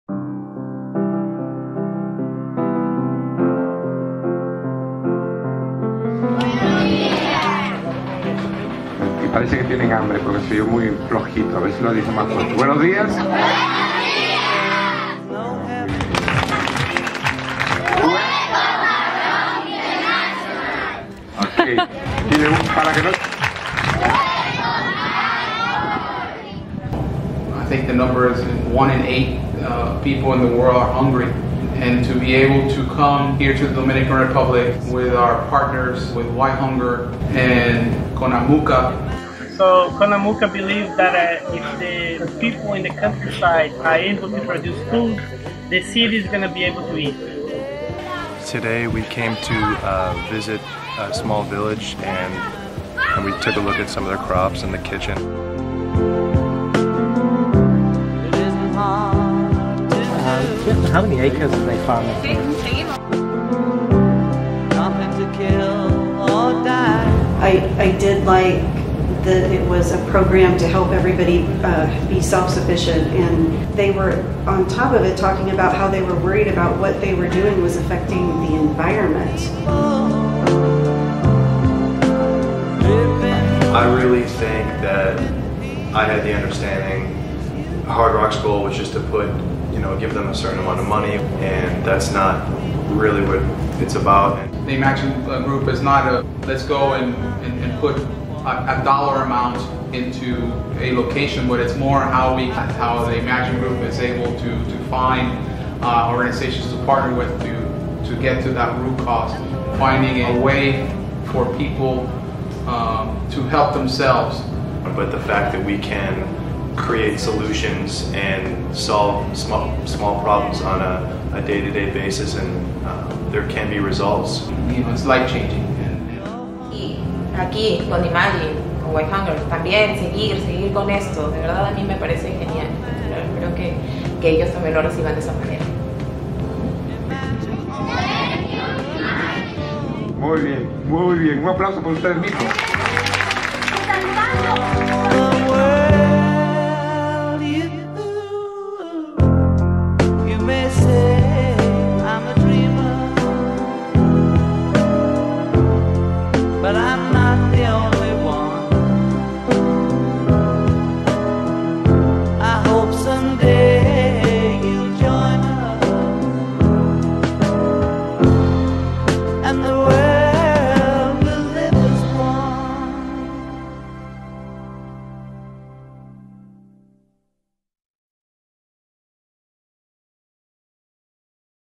Imagine There's No Hunger- CONAMUCA Visit
Dynamic video that shows the work being done around the world to end childhood hunger though Imagine There's